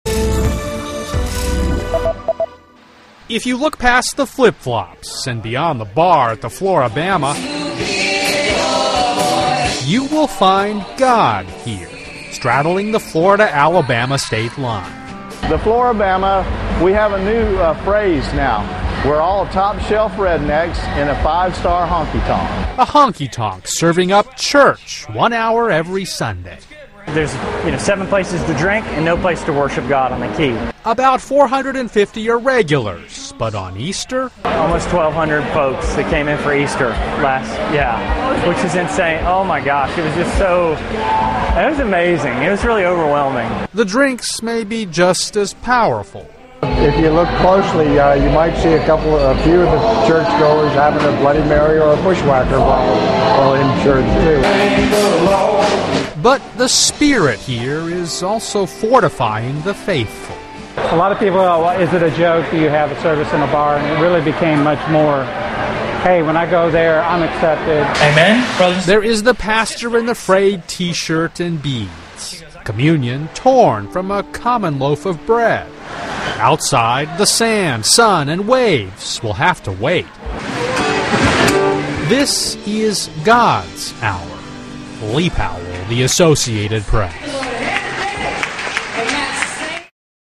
Bibles and Booze Mix at Bar on Sunday Mornings
Bloody Marys, Budweisers and Bibles are an interesting mix on Sunday mornings at the Flora-Bama, an iconic bar straddling the Florida-Alabama state line. Here, church is come-as-you are, flip flops and all. (Aug. 15)